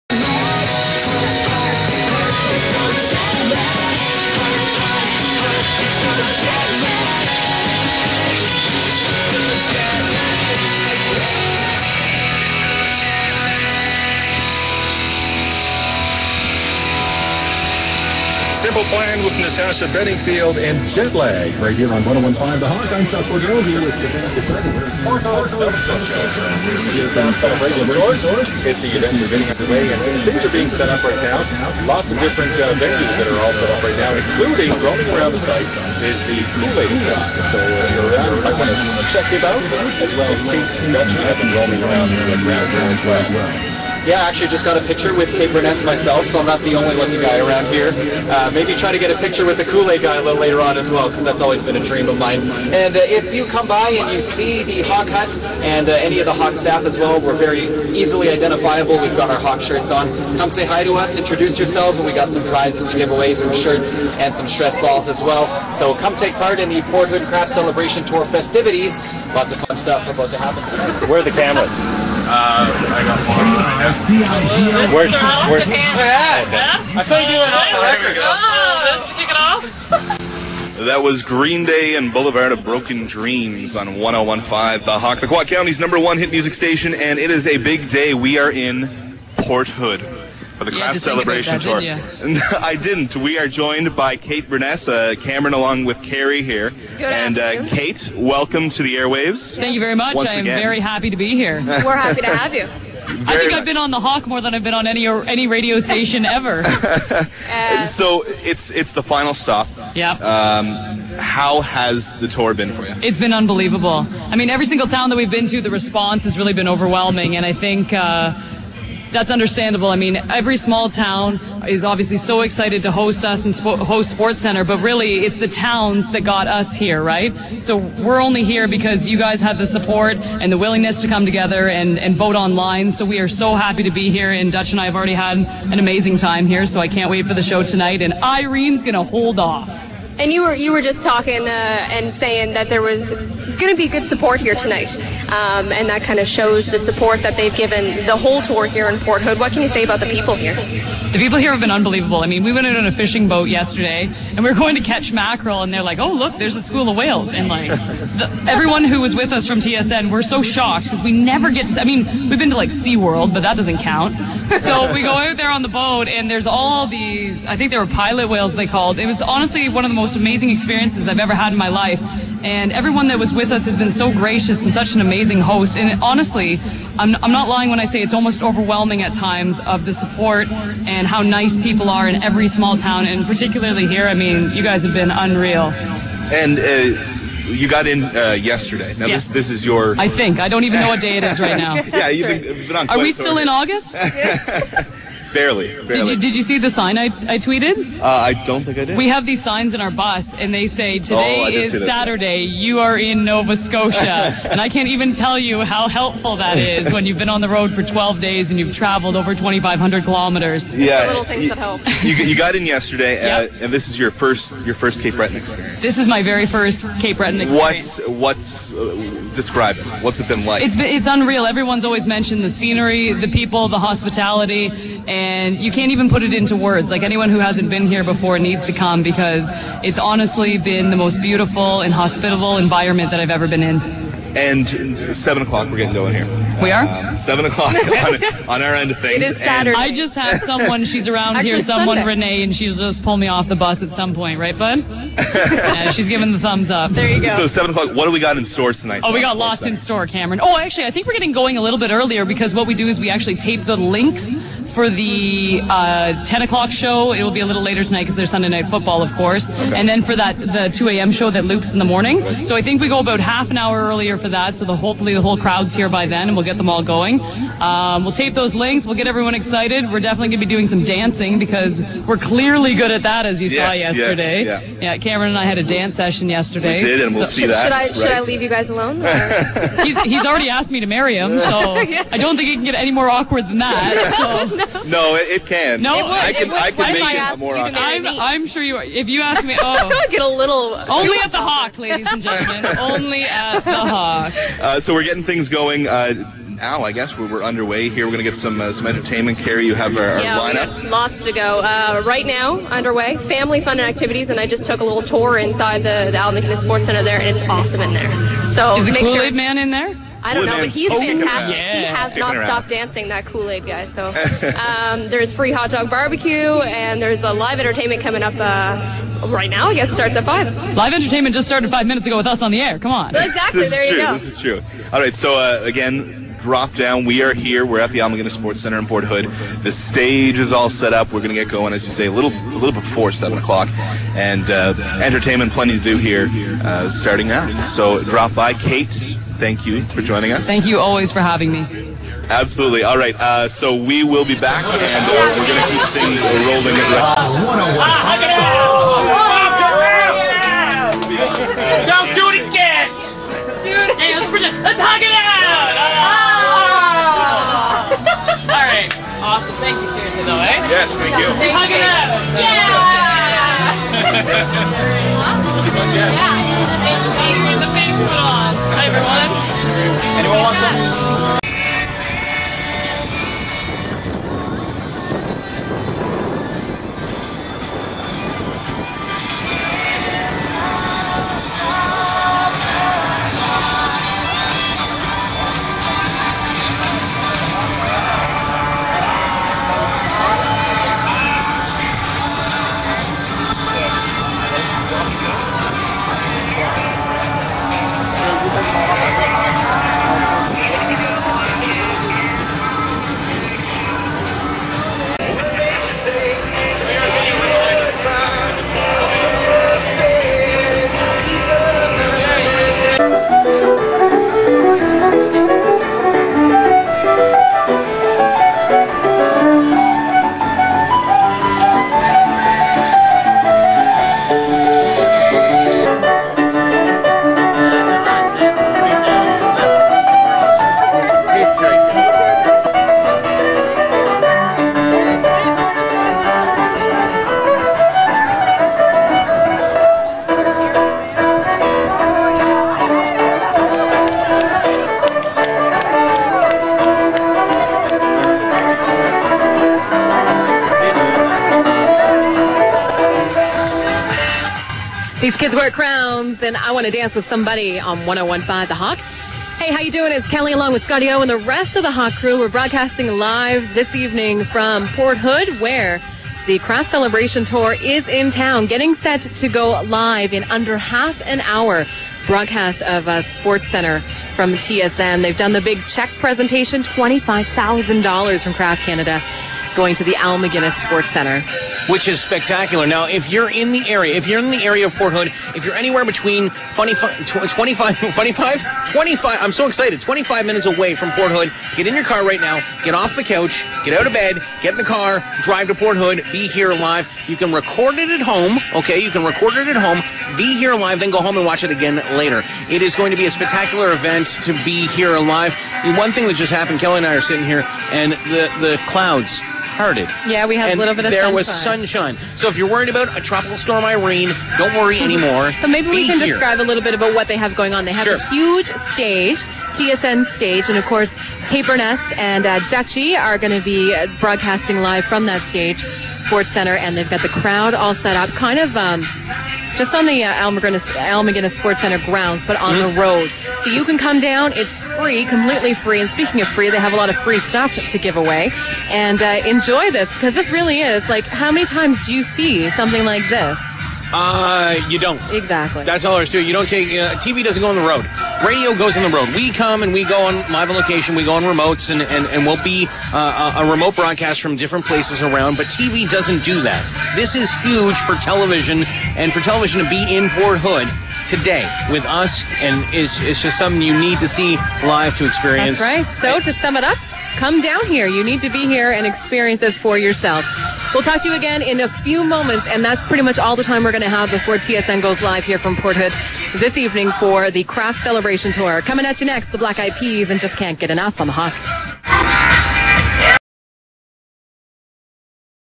Kraft Celebraton Tour-TSN -Hawk LIVE Broadcast Port Hood August 28 2011.mpg
The Kraft Celebration came to Port Hood Nova Scotia on Sunday August 28, 2011. It was the last stop on the tour...10 towns in 10 days. Being one of the 10 towns, Port Hood was awarded a $25 000 cheque from Kraft Canada and they hosted TSN's Sportscentre with Darren Dutchyshen and Kate Beirness live from the community. 1015 The Hawk (fm Radio Station in Port Hawkesbury Nova Scotia) broadcast from the event as a lead-in to the TSN broadcast...here's some highlights including a chat with Kate.